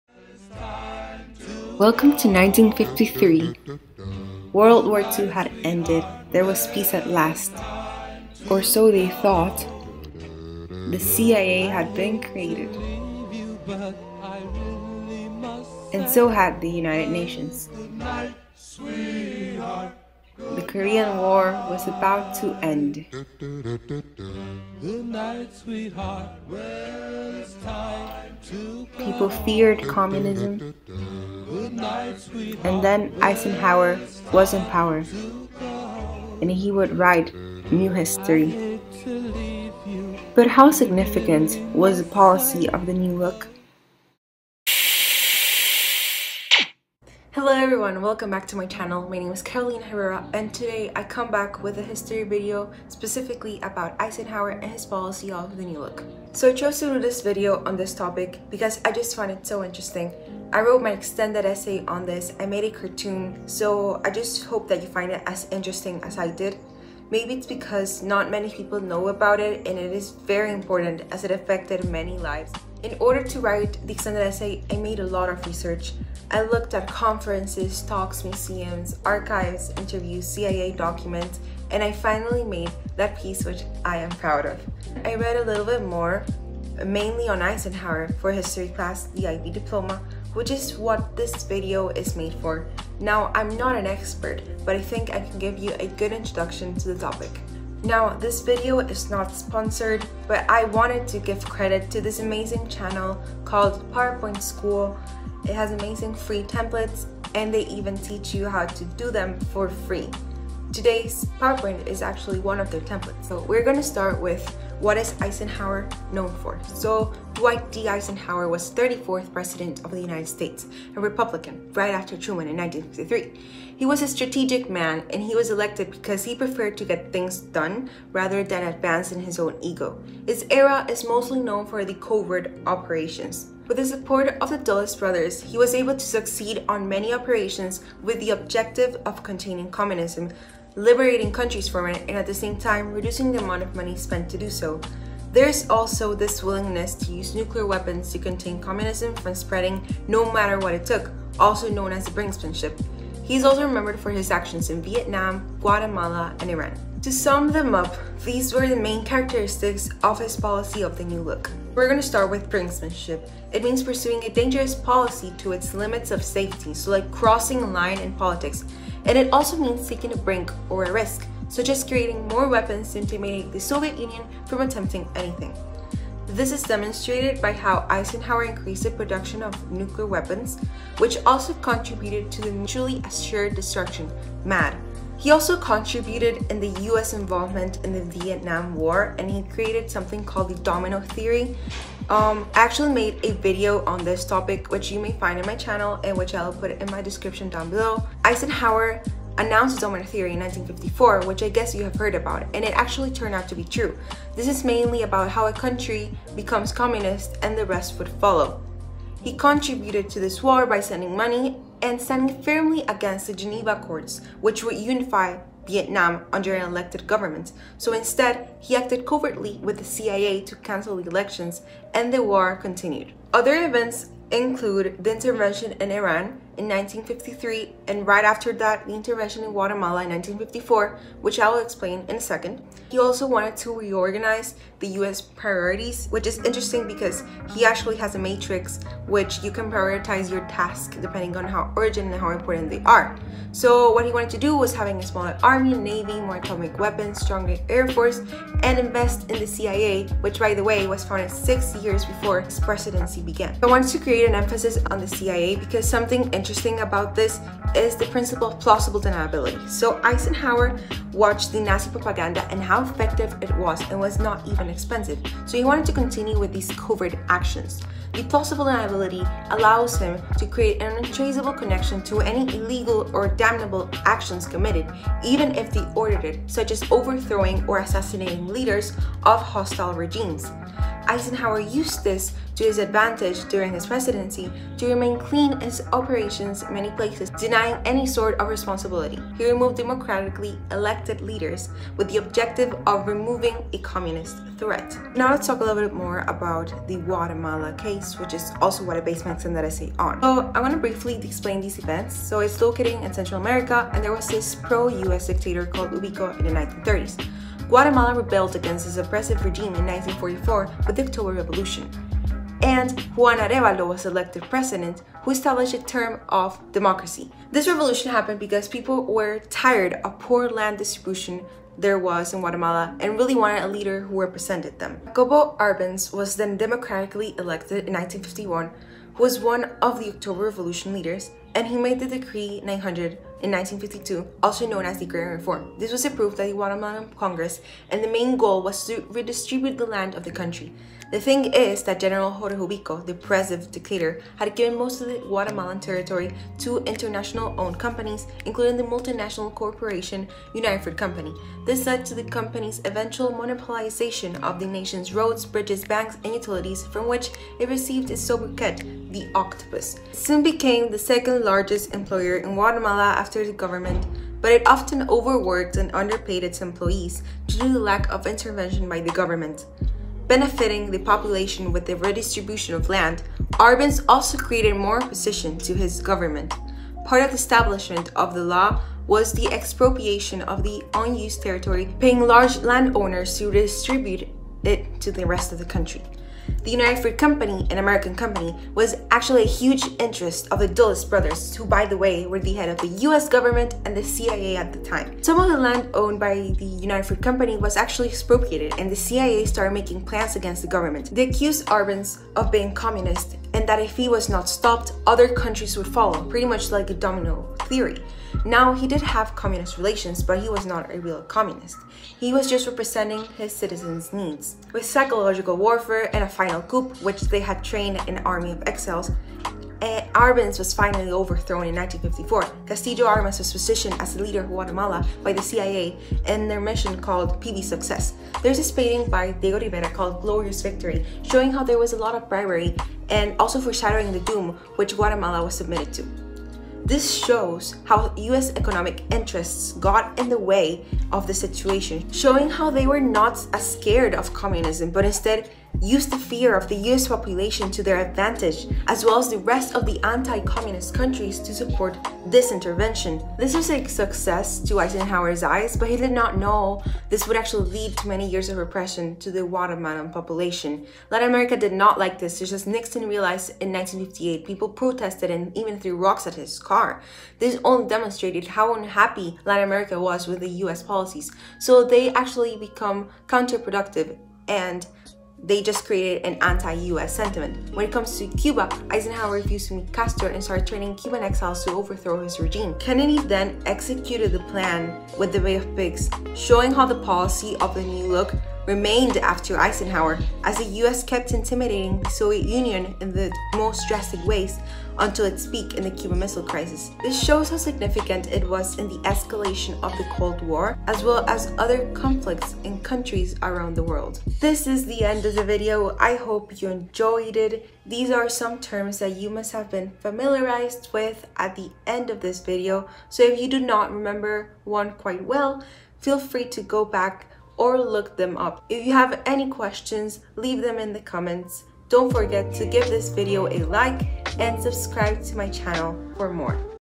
Eisenhower's New Look Policy
Video on the significance of Eisenhower's policy of the New Look 🇺🇸
Bonus case: Guatemala Coup of 1954 🇬🇹

I tried to make this one short but filled with the essential information. I hope you guys like it!

Key terms
2:42  Brinksmanship
3:06  Vietnam War
4:19  CIA and Plausible Deniability
5:41  United Fruit Company
6:30  Intervention in Guatemala